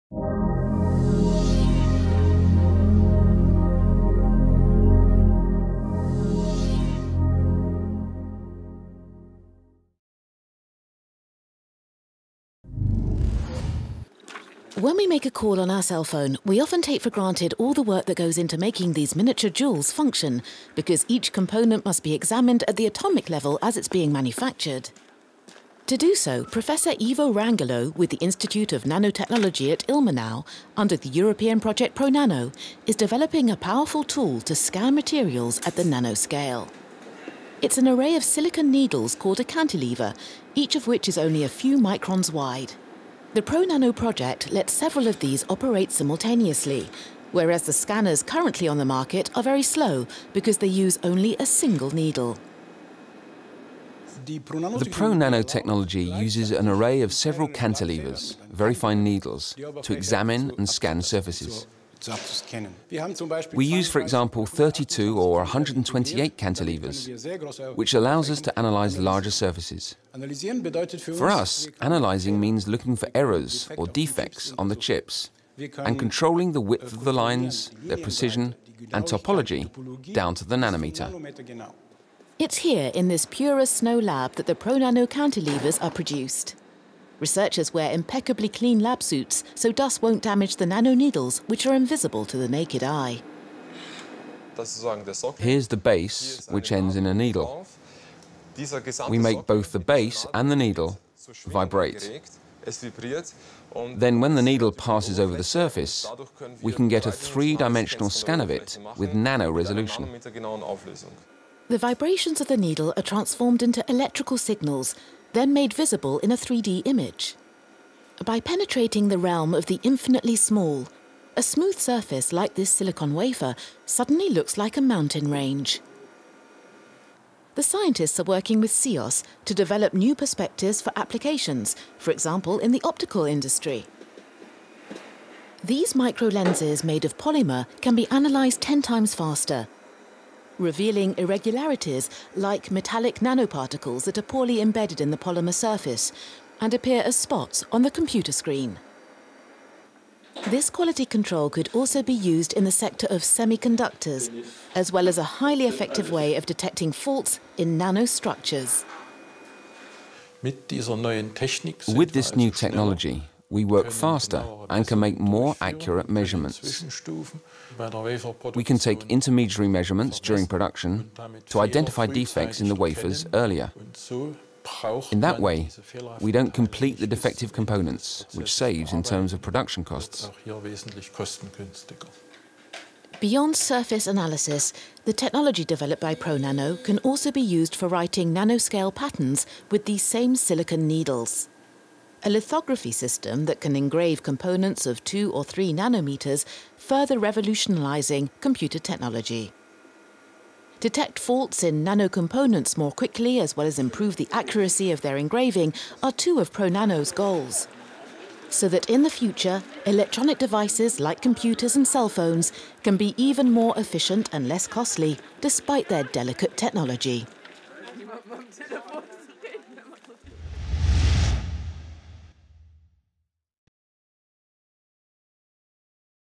ProNano-Massively Parallel Cantilever Probe Platforms for Nanoscale Analysis and Synthesis
Ilmenau University of Technology - Department of Micro- and Nanoelectronic Systems:

Scanning proximity probes (SPP) are uniquely powerful tools for analysis, manipulation and bottom-up synthesis: they are capable of addressing and engineering surfaces at the atomic level and are the key to unlocking the full potential of Nanotechnology.

Current SPP nanotools are limited to single probes with pitifully slow processing rates and, even at the research level, attempts at multiprobe systems have achieved only small arrays and did not find their path into commercialization. This could be a terminal limitation for the future of Nanotechnology, in particular for bottom-up manufacturing, with little prospect for economic throughputs, unless 2-dimensional massively parallel probe arrays can be realised.

Such a development would revolutionise Nanotechnology, triggering an avalanche of new products and processes in a wide range of applications including surface chemistry, materials and the life-health industries. This ground-breaking technology development is the ambitious principal aim of this proposal. Generic massively parallel intelligent cantilever-probe platforms will be produced through a number of existing and ground-breaking techniques.

In a multi-disciplinary approach a technology for fabrication and application of integrated NEMS-chips (Nano Electro Mechanical System) is developed comprising probe realization, electronics and system integration. State-of-the-art micro-fabrication techniques as ultralow energy implantation, vertical through silicon interconnections, flip-chip packaging and ultrasharp tip etching are applied for realizing high performance scanning probe arrays in a scalable layout.

Especially designed integrated cantilever control circuitry, signal multiplexing and intelligent data management allow compact, efficient and scalable electronics for driving the integrated NEMS chips and acquisition of measurement data.

By integration of cantilever array scanning probes and corresponding multi-channel cantilever control electronics into a state-of-the-art laser interferometer controlled large area scanning stage with sub-Nanometer precision a novel and absolutely unique tool for high throughput investigation of the nanocosm will be created.

To validate this novel technology, a series of demonstrations are planned where relevant SMEs will use this technology to carry out sub-10 nm metrology for high throughput manufacturing. Furthermore selected key applications and the results will be used to educate and inform in support of the development of new nanotechnology processes and products.

It is the aim of PRONANO that the new massively parallel scanning probe nanotools with application specific VLSI NEMS (Very Large Scale Integrated Nano-Electro-Mechanical Systems) chips inside should empower nanotechnologists and drive the rapid development of nanoscience, leading to new nanotechnology processes and their industrial exploitation. They will secure the future of nanotechnology with economic throughputs leading to new manufacturing industries.